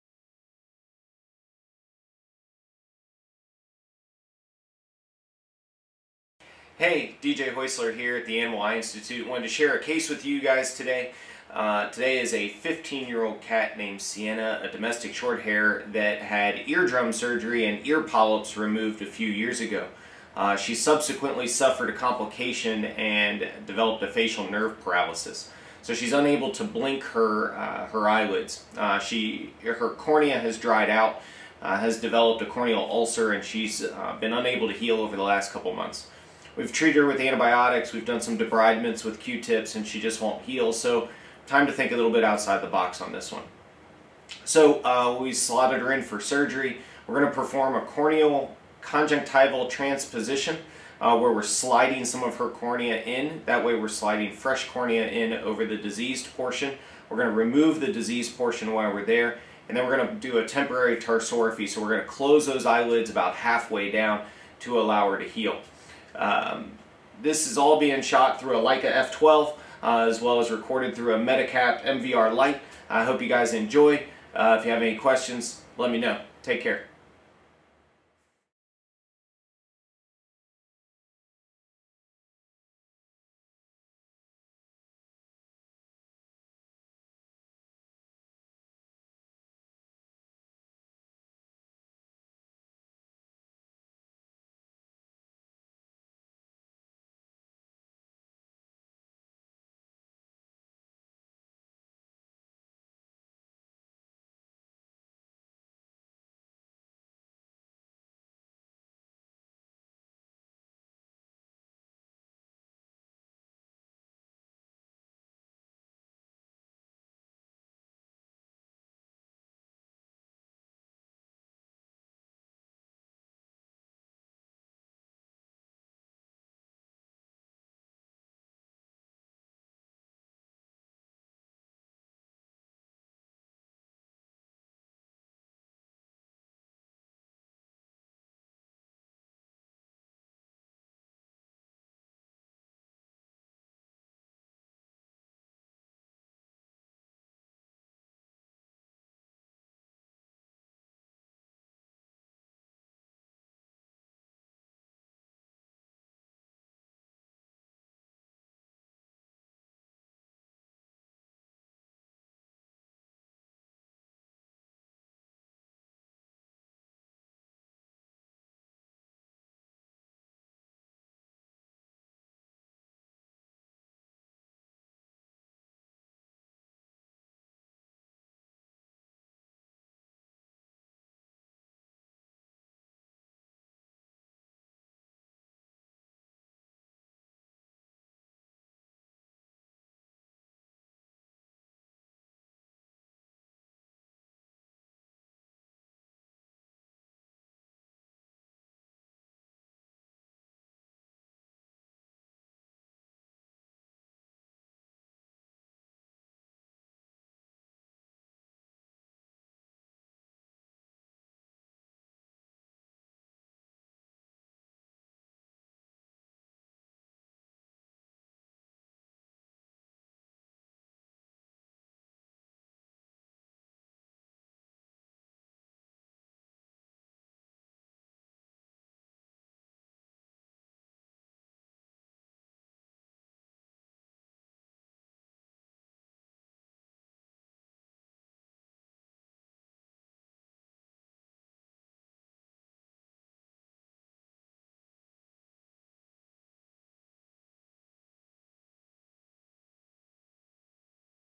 Corneal Conjunctival Transposition (CCT) In An Ulcerated Cat Eye with Facial Nerve Palsy
Check out this awesome surgical video of a corneal conjunctival transposition (CCT) in an ulcerated cat eye with facial nerve palsy secondary to previous ear polyp surgery. See an additional temporary tarsorrhaphy performed which aided in complete resolution of an ulcer that persisted for 3 months.